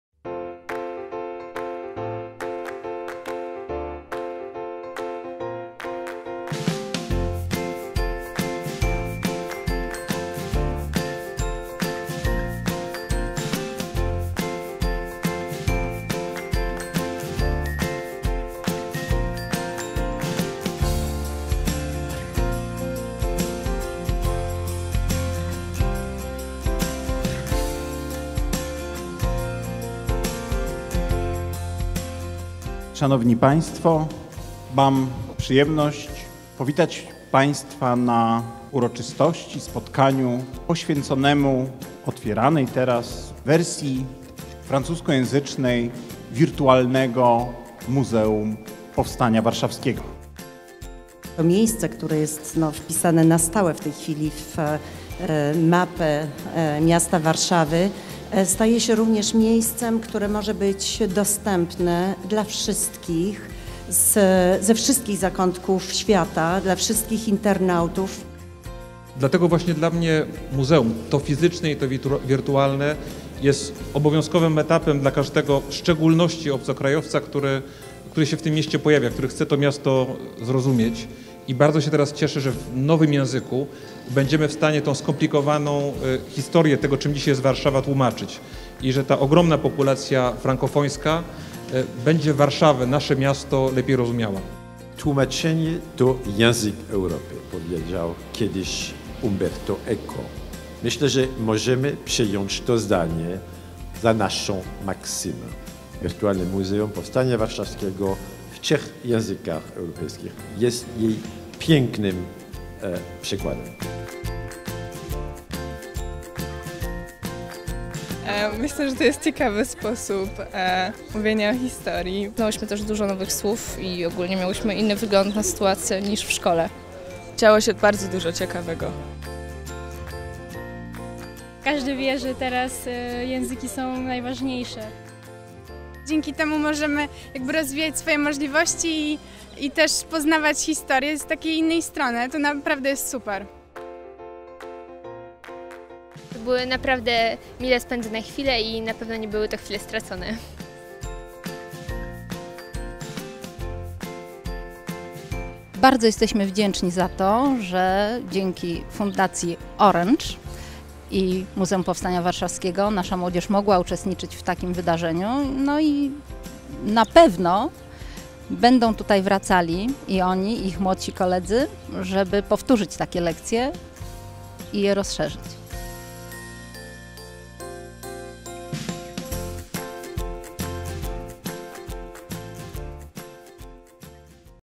Wirtualne Muzeum Powstania Warszawskiego -- maj 2013.
Dzięki współpracy Fundacji Orange i Muzeum Powstania Warszawskiego uruchomione zostały angielska oraz francuska wersje Wirtualnego Muzeum. Serwis to interaktywne narzędzie, dzięki któremu każdy internauta może poznać historię Powstania Warszawskiego. W czasie inauguracji młodzież z LO im. Narcyzy Żmichowskiej w Warszawie uczestniczyła w multimedialnej lekcji historii z wykorzystaniem francuskiej wersji Wirtualnego Muzeum.